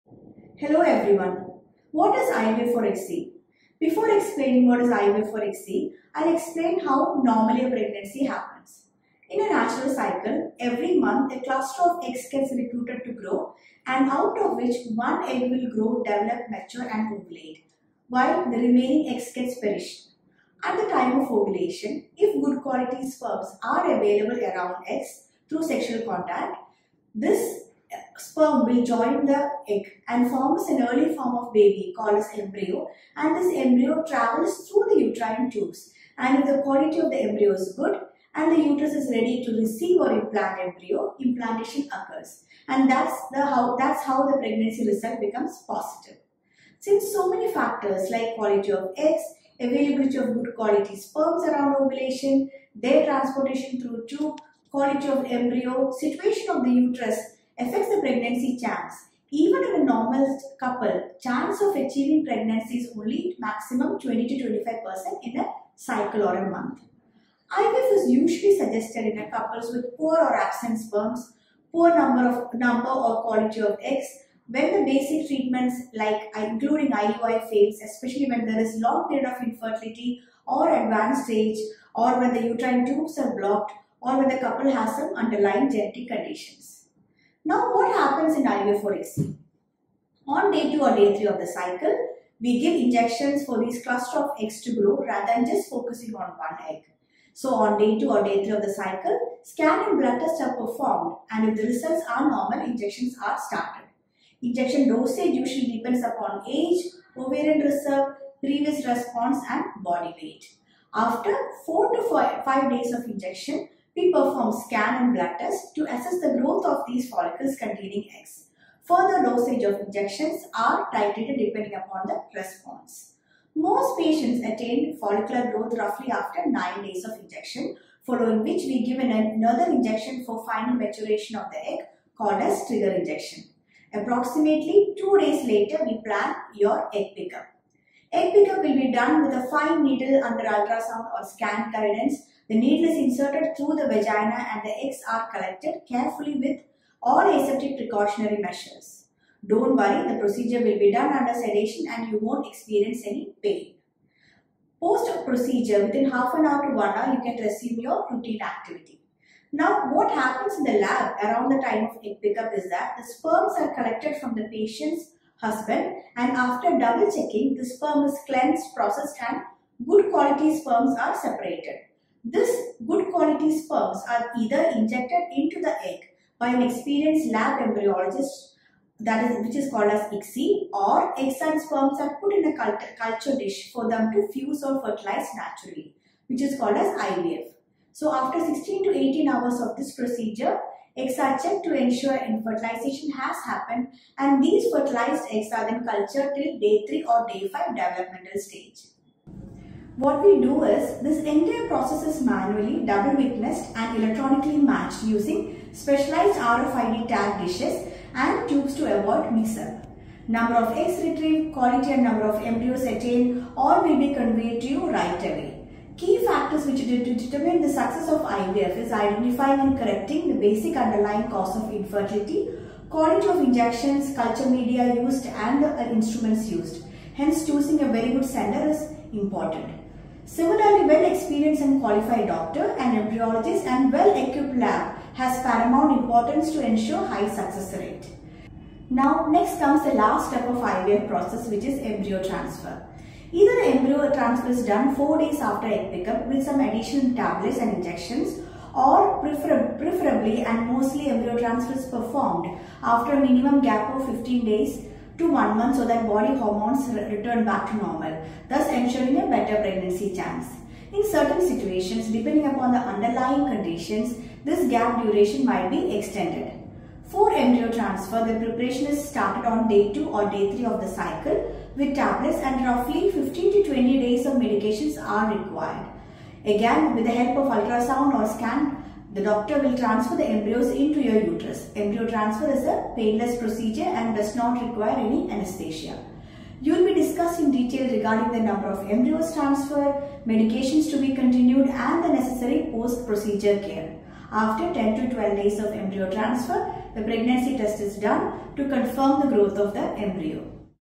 What is IVF or ICSI ? Dr Noushin Abdul Majiyd.
What is  IVF or ICSI? What happens in IVF or ICSI? What is Embryo Transfer ? Dr Noushin Abdul Majiyd, Director CRAFT Hospital telling you some important facts happening in IVF & ICSI treatments.

Dr. Noushin Abdul Majiyd
MD, MRCOG, MRCPI, FRM.
Director, CRAFT Hospitals, Kodungallur, kochi